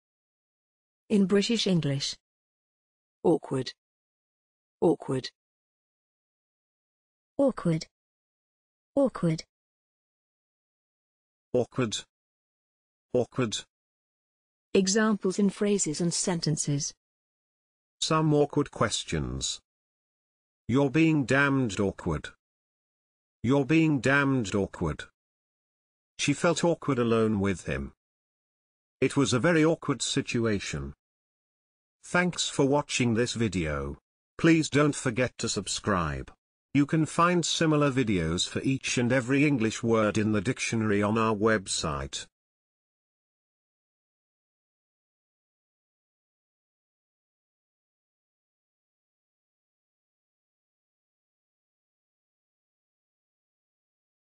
awkward - pronunciation + Examples in sentences and phrases
This is the pronunciation of awkward in British English + a few examples of its pronunciation in phrases and sentences.